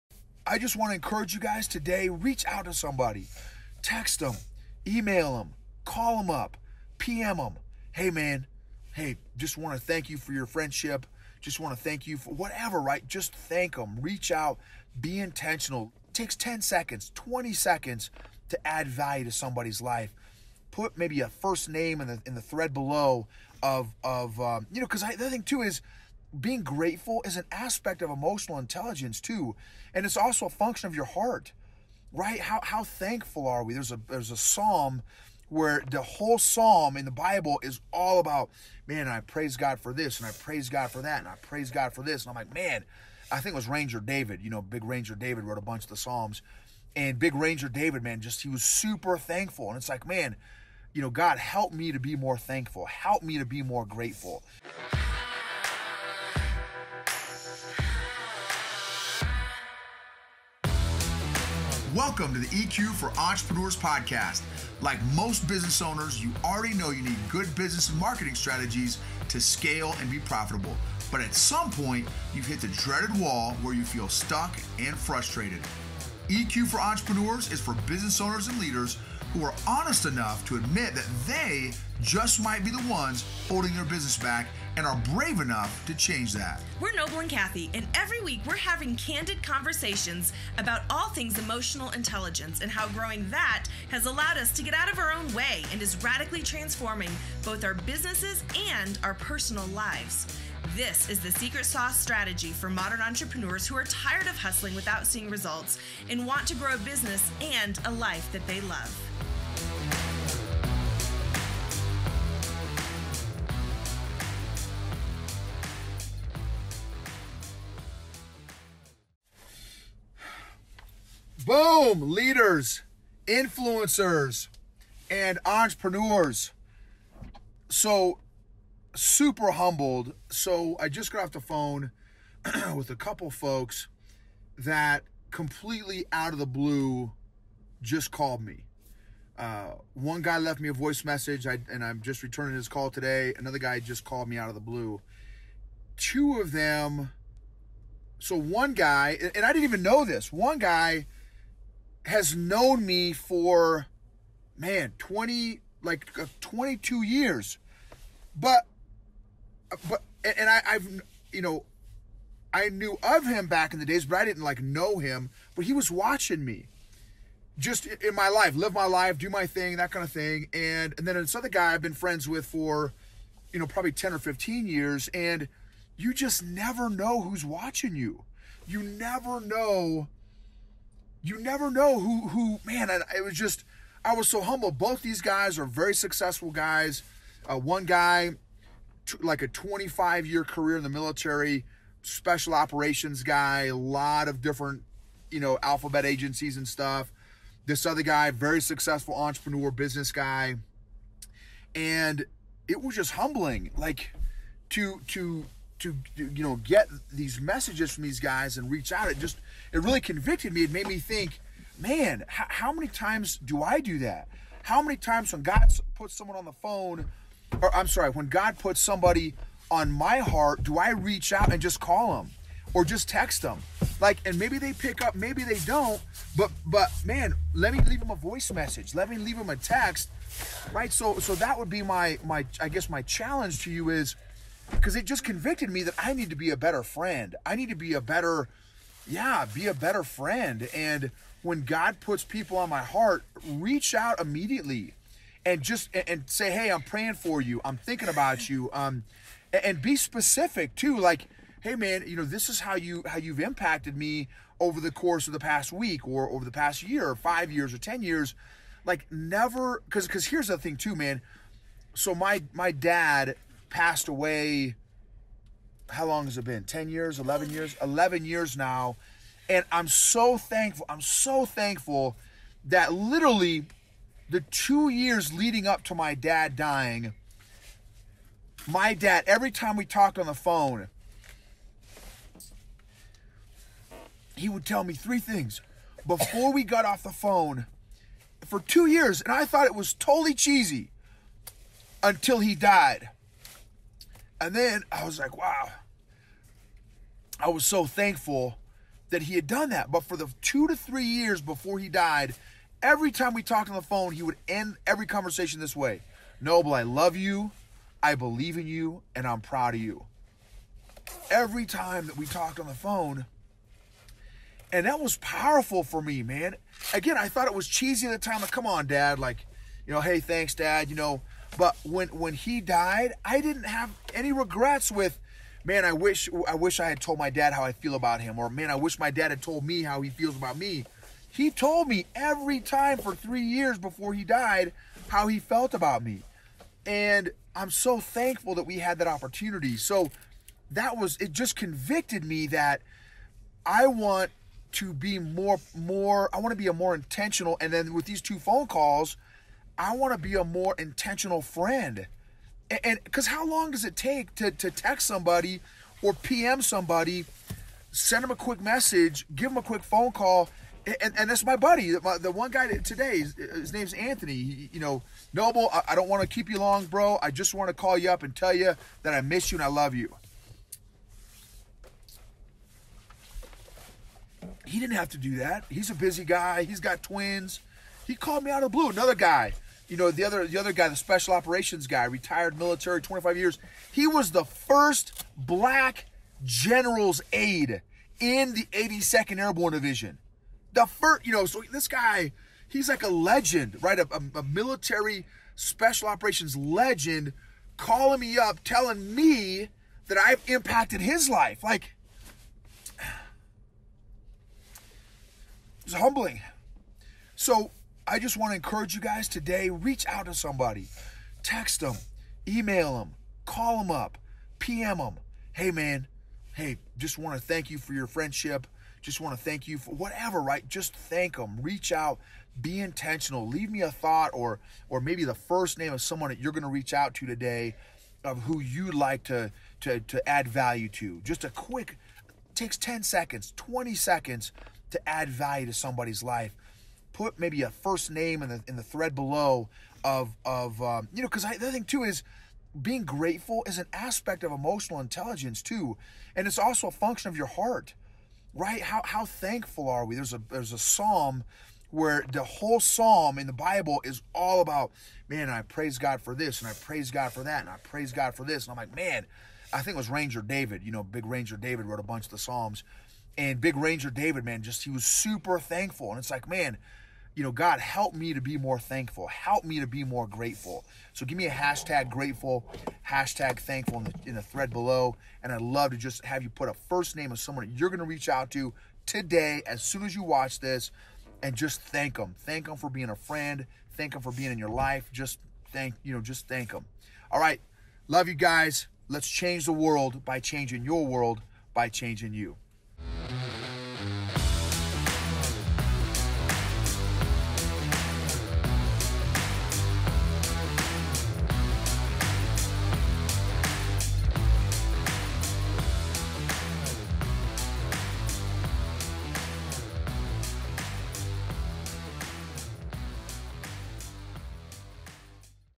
Episode 113: Who Are You Thankful For? Who Is Thankful for You?
2 calls from out of the blue

You never know who is watching you & how you’re living your life.

How often do you let your friends know that you’re thankful for them?

How good of a friend are you?

How can you be a better friend?

Be specific w/how someone has impacted you.

2 years before my dad died, every time he & I talked on the phone, he would tell me 3 things:
I’m proud of you.

How thankful are you?

Thankfulness is helpful for our own emotional health